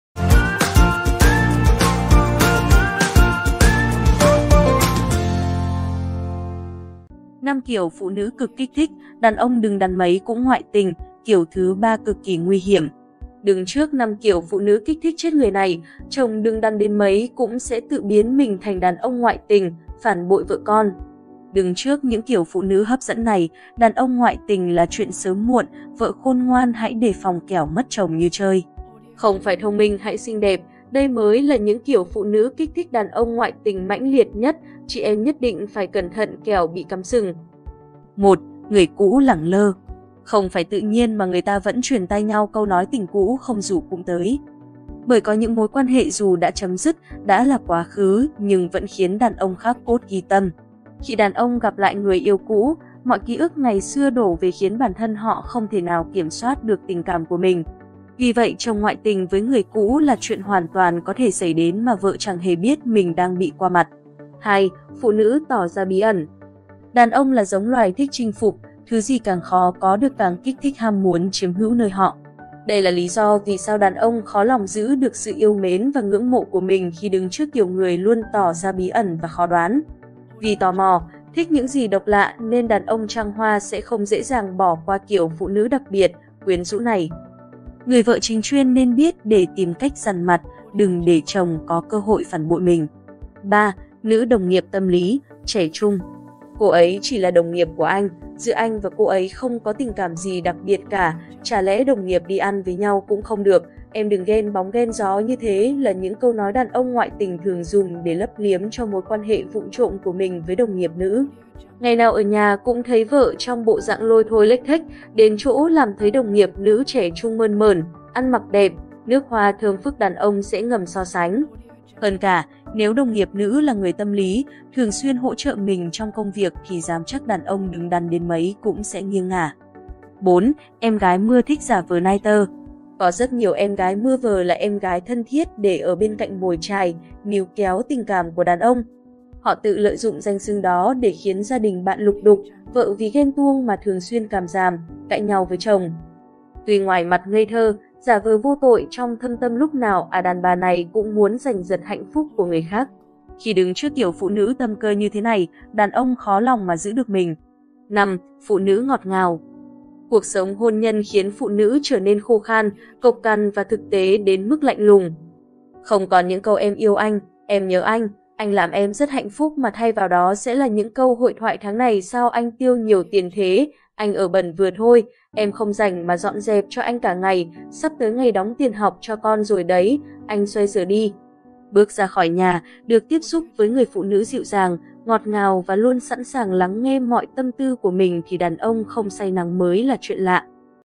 5 kiểu phụ nữ cực kích thích, đàn ông đứng đắn mấy cũng ngoại tình, kiểu thứ ba cực kỳ nguy hiể...
5 kiểu phụ nữ cực kích thích, đàn ông đứng đắn mấy cũng ngoại tình, kiểu thứ ba cực kỳ nguy hiểm - Blog Radio tâm sự

5 kiểu phụ nữ cực kích thích, đàn ông đứng đắn mấy cũng ngoại tình, kiểu thứ ba cực kỳ nguy hiểm Đứng trước 5 kiểu phụ nữ 'kích thích chết người' này, chồng đứng đắn đến mấy cũng sẽ tự biến mình thành đàn ông ngoại tình, phản bội vợ con.